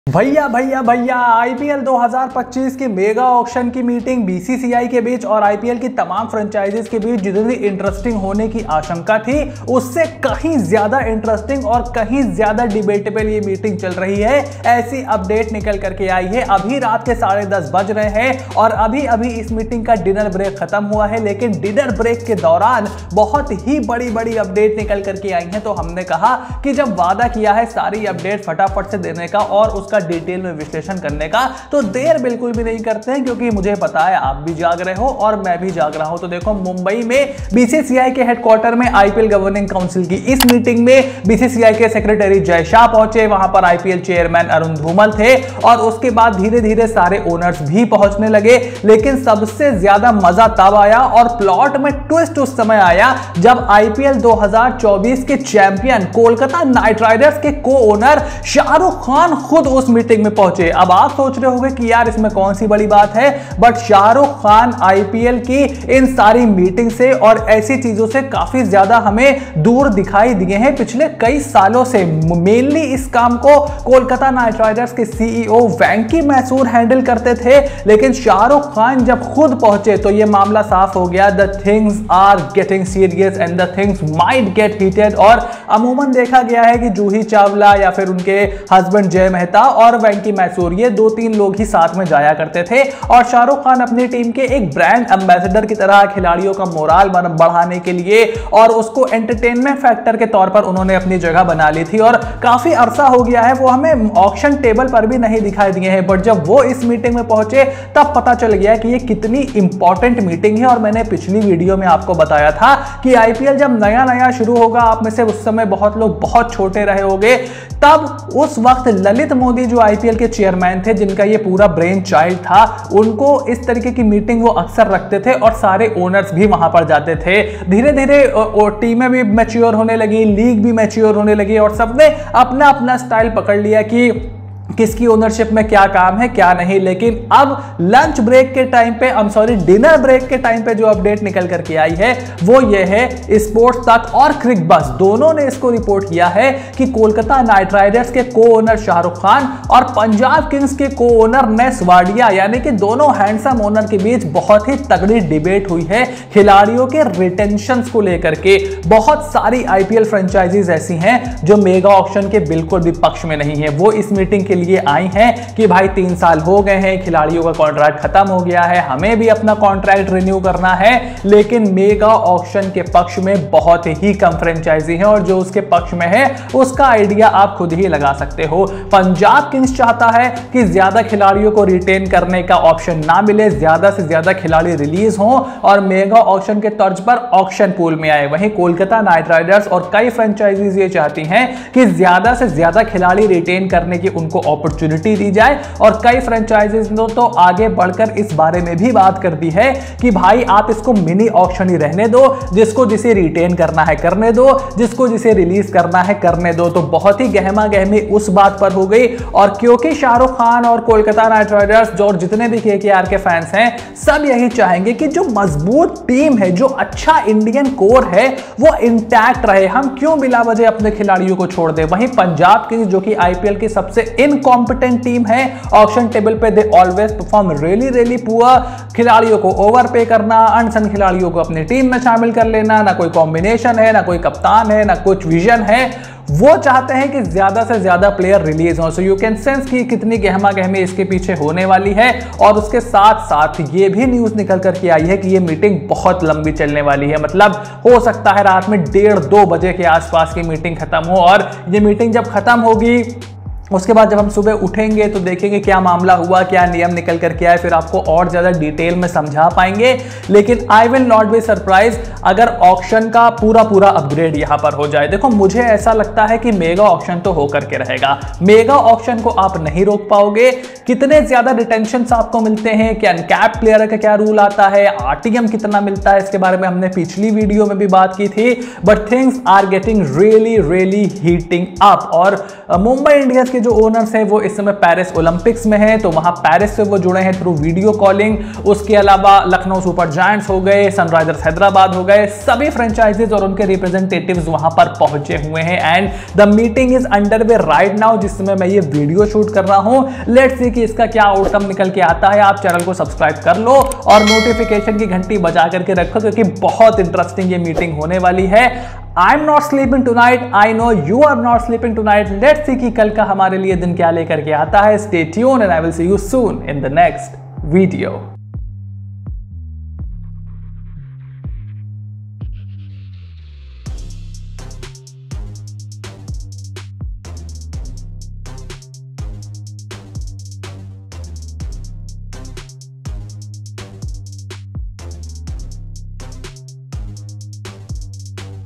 BREAKING : KKR Owner Shahrukh Khan and PBKS Owner Ness Wadia BIG FIGHT😱 in BCCI Meeting for IPL 2025
According to a top BCCI source who was part of the meeting, Shah Rukh was fiercely making the case against mega auctions. At one stage, the KKR owner was also believed to be engaged in a heated argument with Ness Wadia, the co-owner of Punjab Kings, over number of retentions; Shah Rukh was in favour of major retentions while Wadia is believed to be against too many of them.

Looking forward to hear from you. Thank you for your support !